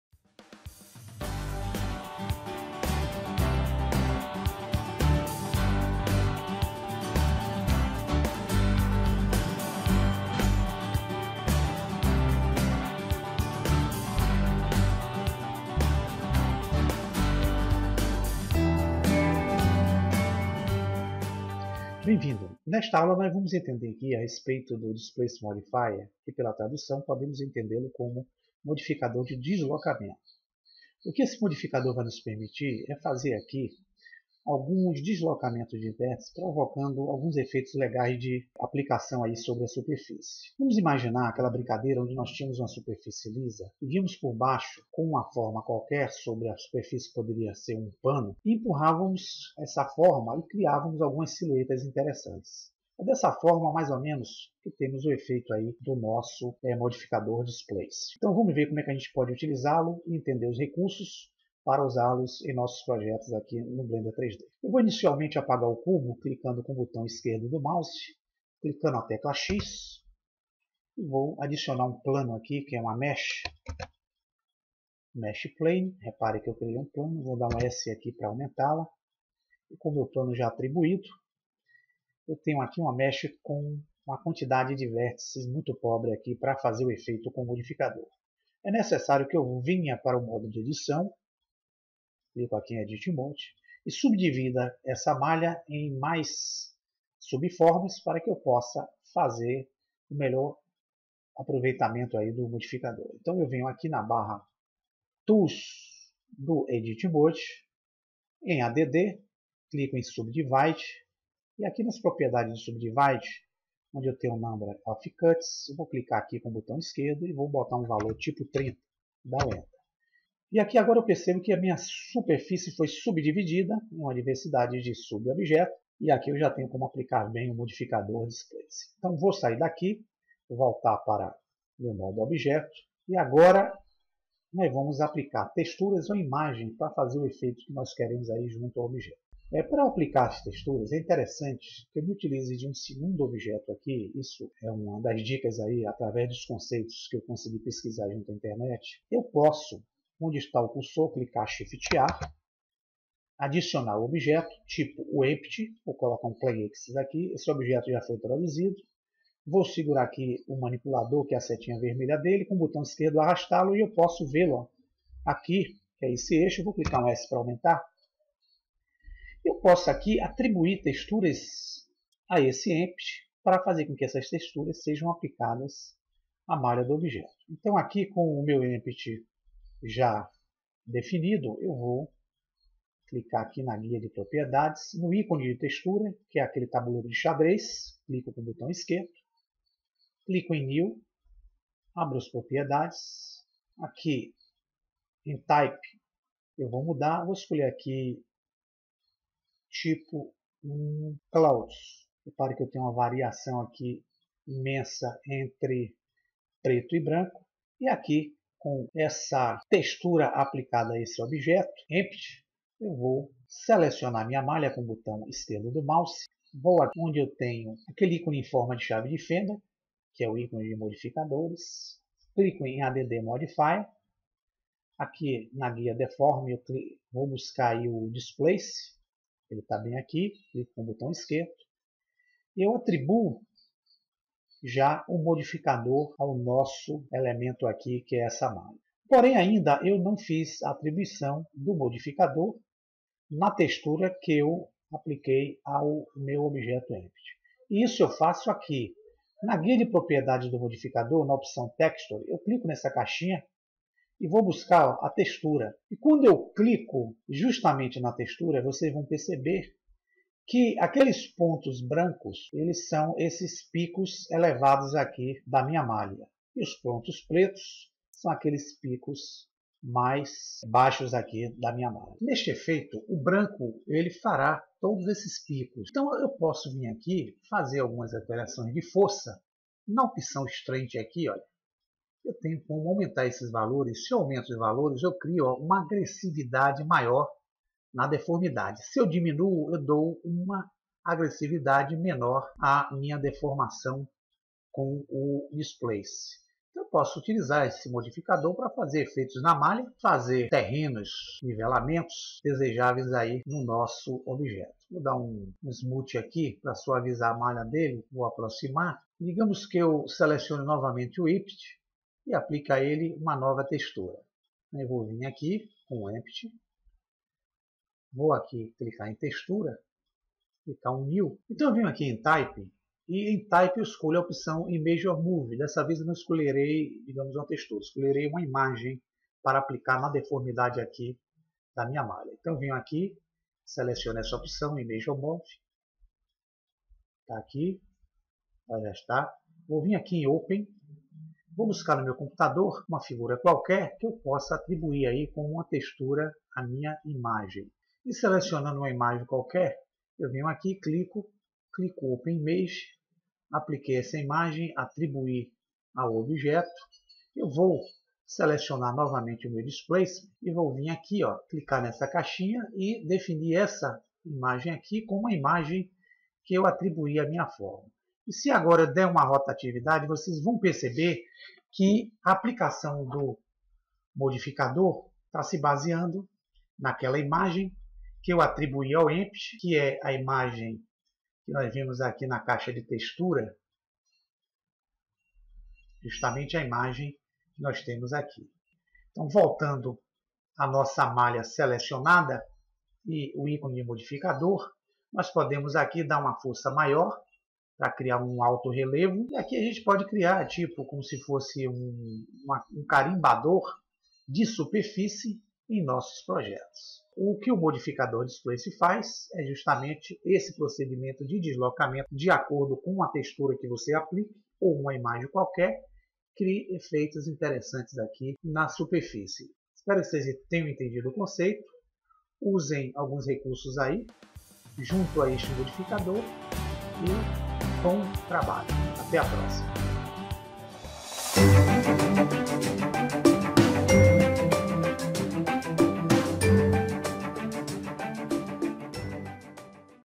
Blender # 61 - MODIFICADOR DISPLACE
Noções básicas de uso do DISPLACE MODIFIER que permite transformar as vértices de uma malha, criando em decalques, efeitos interessantes.